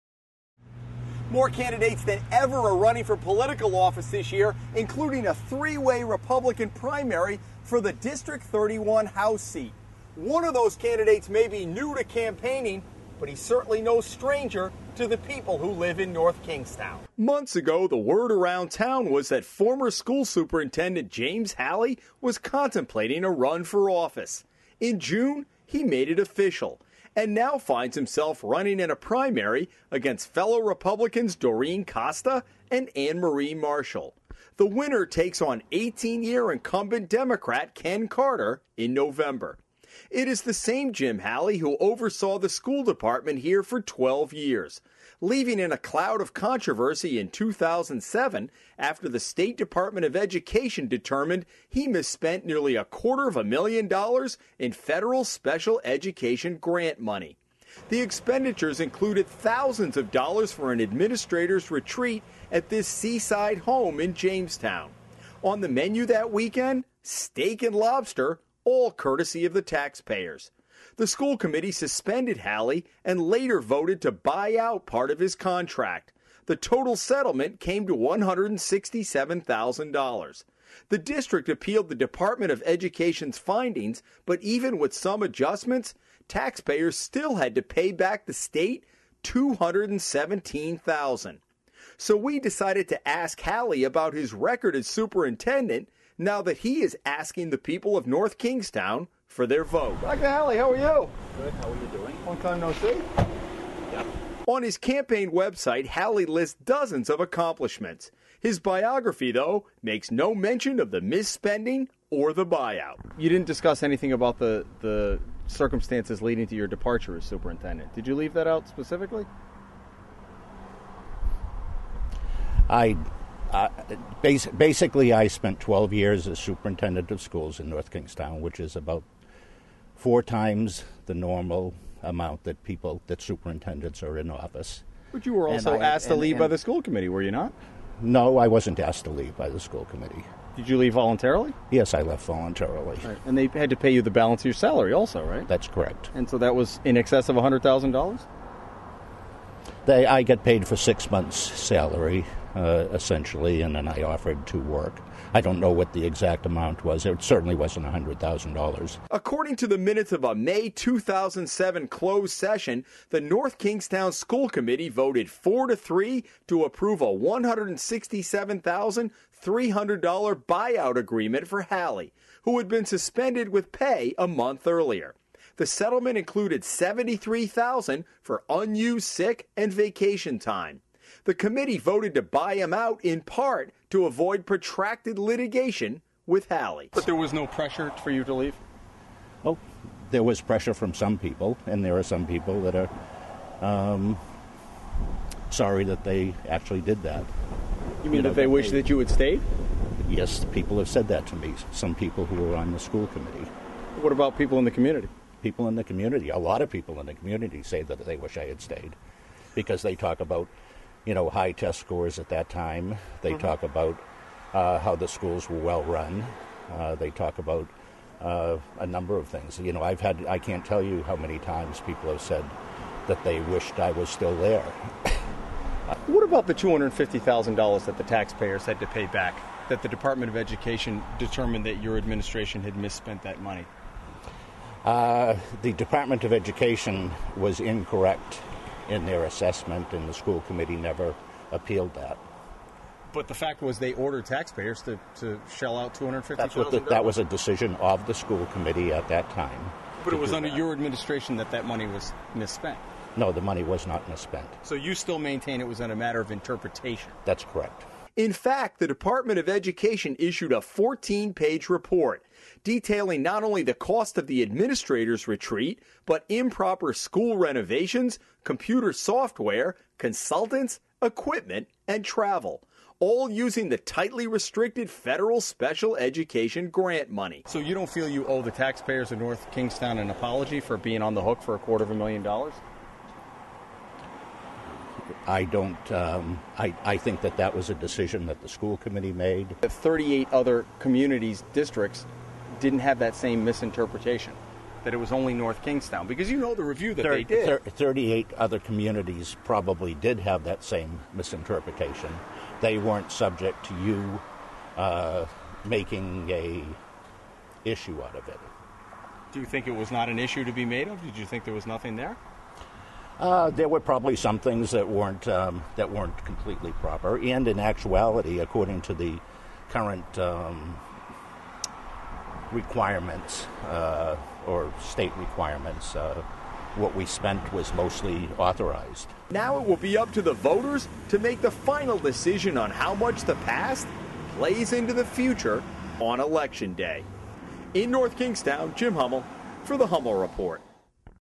A Familiar Face
Three years ago North Kingstown's school superintendent was forced out by the school committee under a cloud of controversy. The state Department of Education determined his administration improperly spent nearly a quarter of a million dollars of restricted special education grant money.  Now, James Halley is back, asking the voters of  North Kingstown to send him to the General Assembly.  This week, Jim Hummel catches up with the candidate.